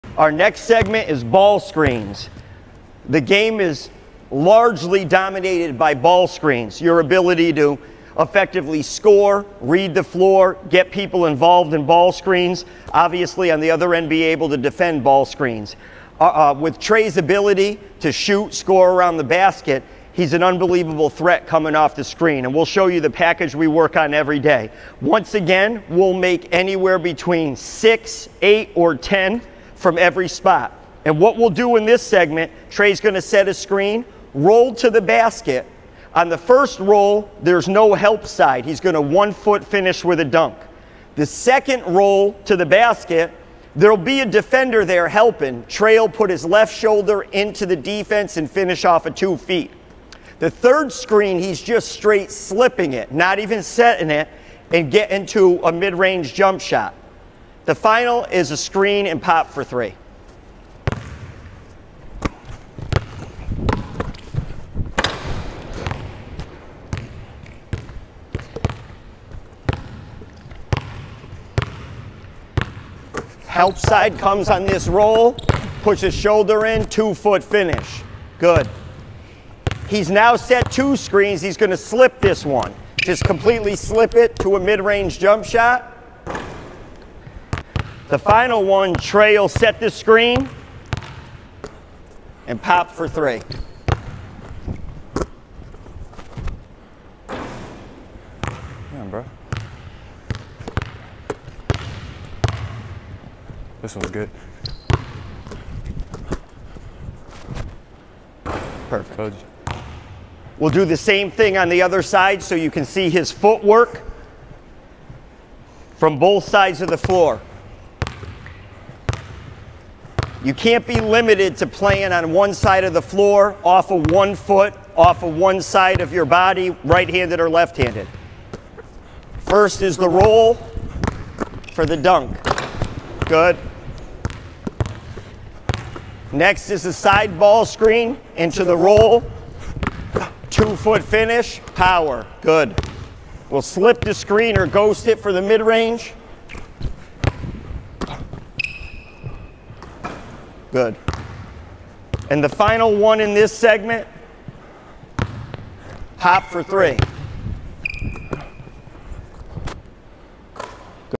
Basketball Ball Screen Series with Texas Forward Tre Mitchell!
with Tre Mitchell,
University of Texas Men's Basketball Player;
played at the University of Massachusetts as a freshman and sophomore;
averaged 18 points and 7.2 rebounds per game at UMass;
2020-21 Atlantic 10 All-Conference First Team; 2020-21 Atlantic 10 All-Academic Team;
2019-20 Atlantic 10 Rookie of the Year; 2019-20 Atlantic 10 All-Conference Second Team;
first UMass player ever to be First Team All Conference and First Team All Academic;
2019 Connecticut Gatorade Boys Basketball Player of the Year in high school; 2x Power 5 Conference AAA Player of the Year; 2x Hoophall Classic Most Valuable Player

Step up your game with an offensive workout from Tre Mitchell!

Get better at dribbling with both hands to be able to attack in either direction and take advantage of the defense!

Develop shooting skills through a variety of drills that get reps from spots you'll likely shoot from during games!

Work on moves in the post area to develop versatility and convert more high percentage shots!